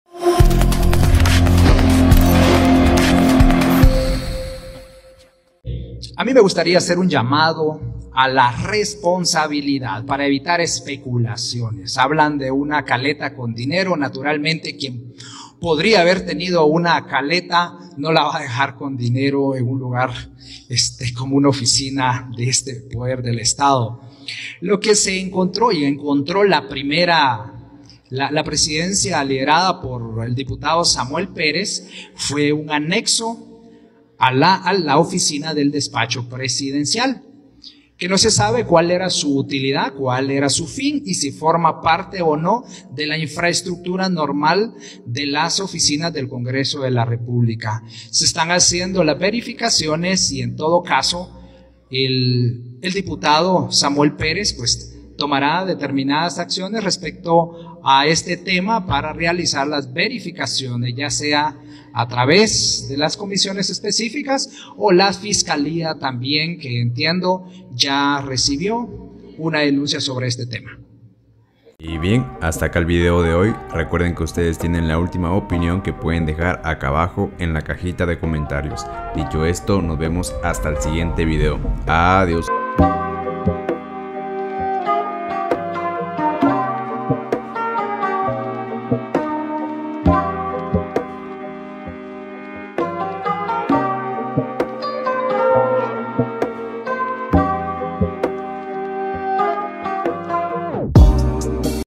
INSÓLITO! DESCUBREN UNA CALETA CON POSIBLE DINERO EN UNA PARED DEL CONGRESO, GUATEMALA
Nuestro Objetivo🏁
 OBJETIVO
100.000✅
200.000✅
300.000✅
400.000✅
500.000✅
600.000✅
700.000✅
800.000✅
900.000✅